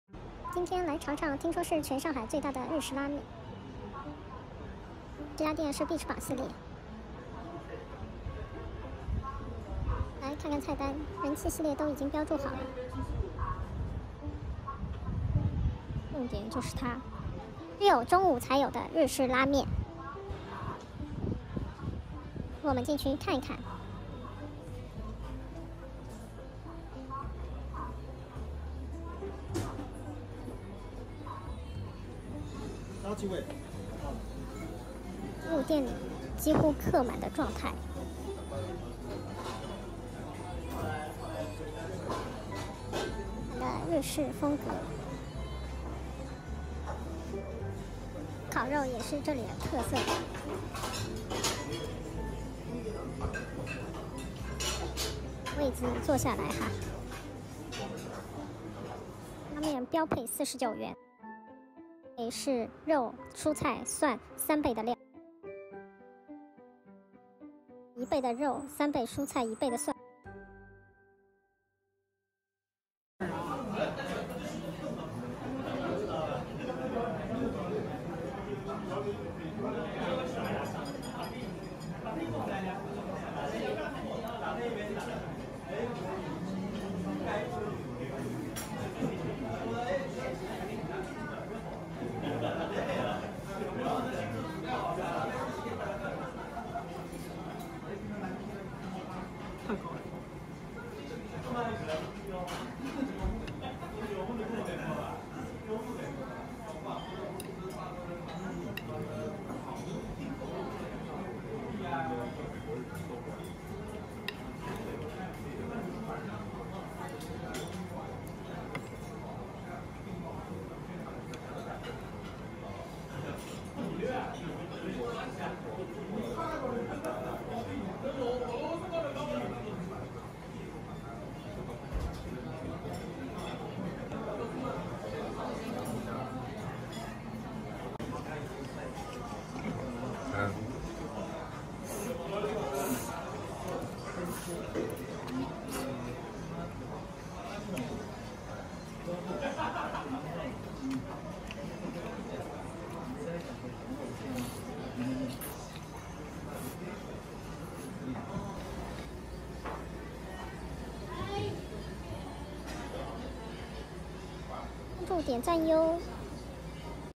上海美食探店—巨大大大日式拉面！日式拉面
看到网上有推荐上海最大的日式拉面~
周末打算去尝一尝
店面不大，开在商务办公地方，比较隐蔽~
人气十分旺，几乎客满的状态~
店里主打的日式拉面只有中午才供应~
要去的小伙伴，记得中午去哈~
标配的拉面是49元一碗，看到别人点，也十分料足~
顶配是肉、蔬菜、蒜，3倍的量~
视频里这碗，只是一倍肉、蒜、3倍蔬菜，
一上来真的蛮震撼的~
汤底浓郁，面条很劲道，蔬菜很入味~
唯一不足是肉有点厚了，个人不是很喜欢~
下次可以多加一份面哈~
如果大家喜欢我的视频，
欢迎评论区留言！

I saw that there is a recommendation on the Internet for the largest Japanese ramen in Shanghai~
Going to try it this weekend
The store is not big, it is located in a business office, and it is relatively hidden~
The popularity is very strong, and it is almost full~
The main Japanese ramen in the store is only available at noon~
Friends who are going, remember to go at noon~
The standard ramen is 49 yuan a bowl, and seeing others order it, it is also very expected~
Top with meat, vegetables, garlic, 3 times the amount~
The bowl in the video is just twice the meat, garlic, and three times the vegetables.
It was really shocking when it came up~
The soup base is rich, the noodles are very strong, and the vegetables are very tasty~
The only downside is that the meat is a bit thick, I don't like it very much~
I can add more noodles next time~
Welcome to leave a message in the comment area!
Remember to follow, retweet, and like! ❤